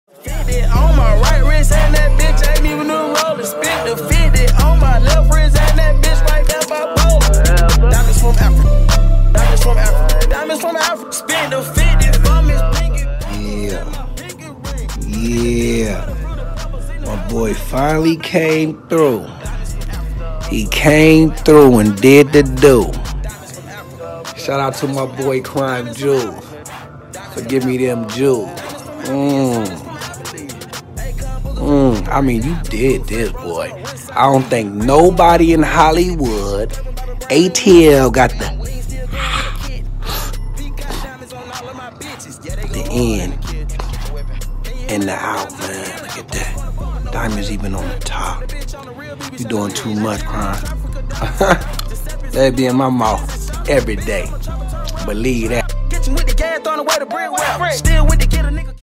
Jackie Long x Crimes Jewels
Actor Jackie Long joins the VVS Gang, becoming the first actor in HollyWood to own a Crimes Jewels' Fully Loaded Custom Front to Back Ice Grill. Create History.